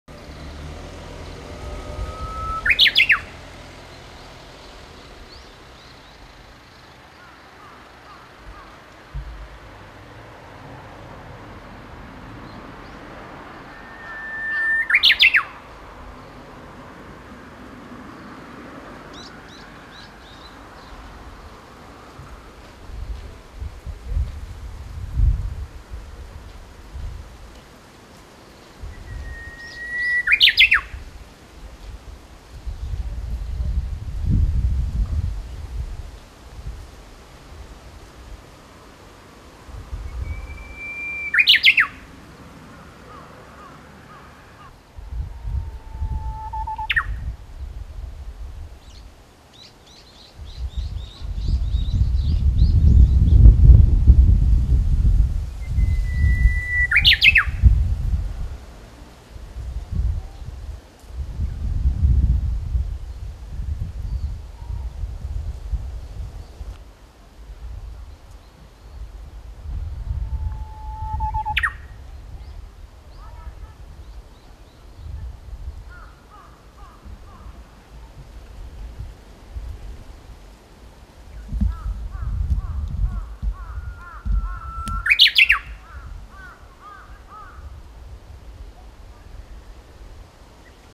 ウグイスの鳴く姿
春の訪れを告げるウグイスの鳴く姿をカメラがとらえた